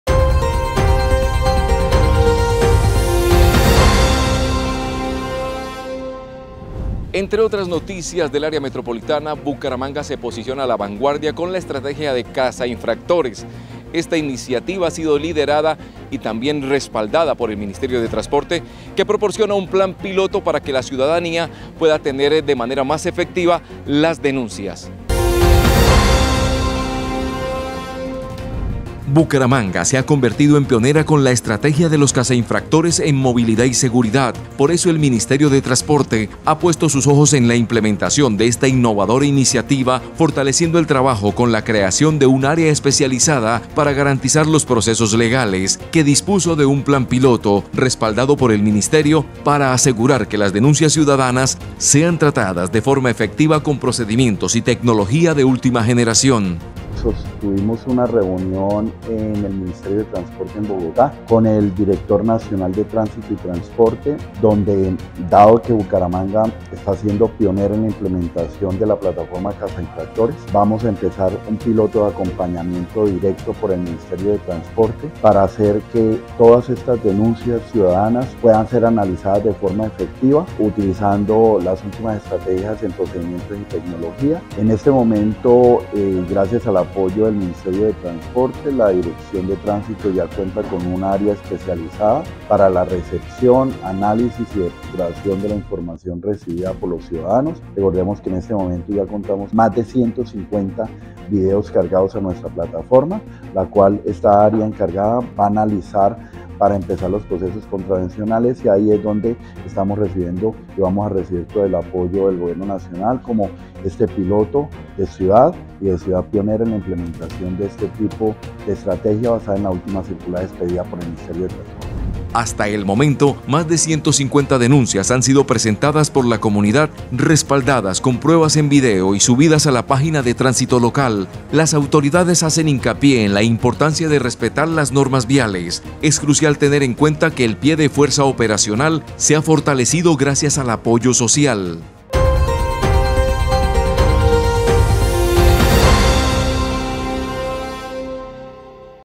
𝑩𝒖𝒄𝒂𝒓𝒂𝒎𝒂𝒏𝒈𝒂 lidera la revolución en seguridad vial con los ‘𝒄𝒂𝒛𝒂-𝒊𝒏𝒇𝒓𝒂𝒄𝒕𝒐𝒓𝒆𝒔’
🚗  𝑩𝒖𝒄𝒂𝒓𝒂𝒎𝒂𝒏𝒈𝒂 lidera la revolución en seguridad vial con los ‘𝒄𝒂𝒛𝒂-𝒊𝒏𝒇𝒓𝒂𝒄𝒕𝒐𝒓𝒆𝒔’ 🚗. El 𝑴𝒊𝒏𝒊𝒔𝒕𝒆𝒓𝒊𝒐 𝒅𝒆 𝑻𝒓𝒂𝒏𝒔𝒑𝒐𝒓𝒕𝒆 respalda la innovadora estrategia de los “caza-infractores” en Bucaramanga. La ciudad se coloca a la vanguardia en seguridad vial, comprometida con la mejora continua en este importante ámbito. ¡Descubre cómo están transformando las normas de tráfico!.